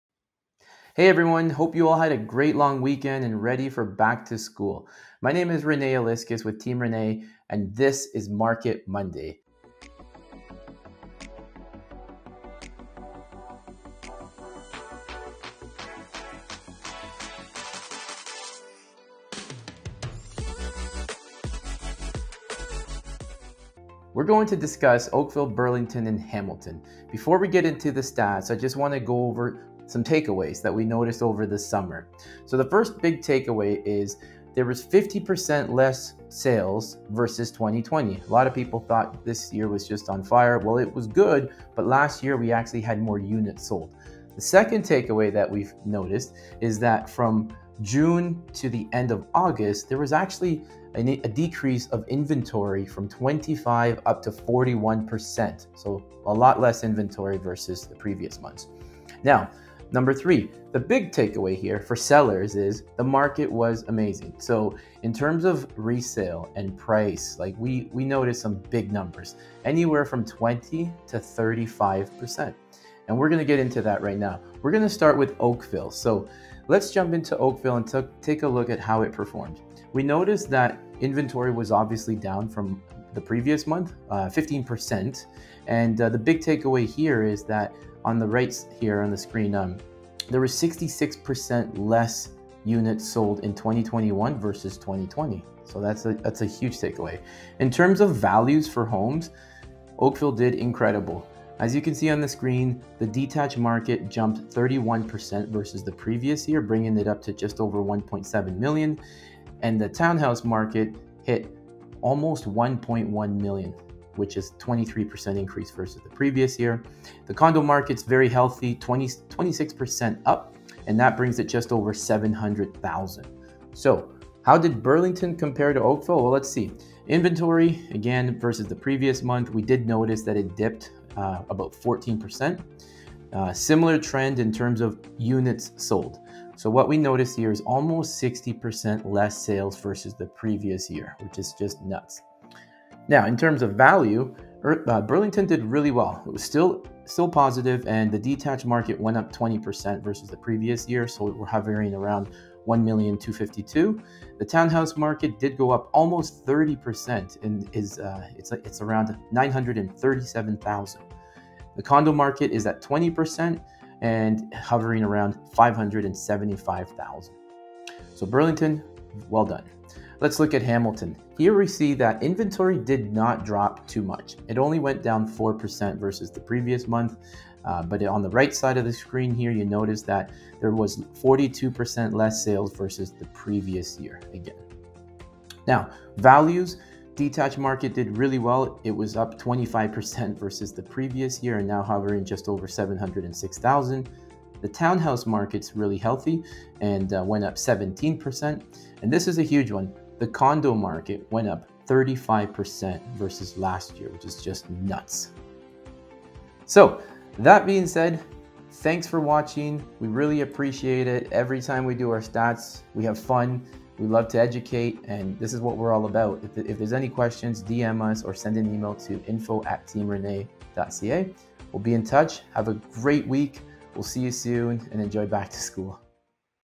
August 2021 Stats | Team Rene | Market Monday
Rene Illescas from Team Rene discusses market updates for August 2021

TEAM RENE | RE/MAX Aboutowne Realty Corp. Brokerage